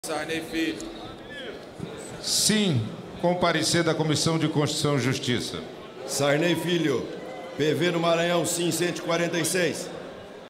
Como cuspir um voto
Deputado Sarney Filho deixa Ministério do Meio Ambiente para votar, sem vontade, a favor da interrupção da investigação contra o Governo que lhe deu o cargo.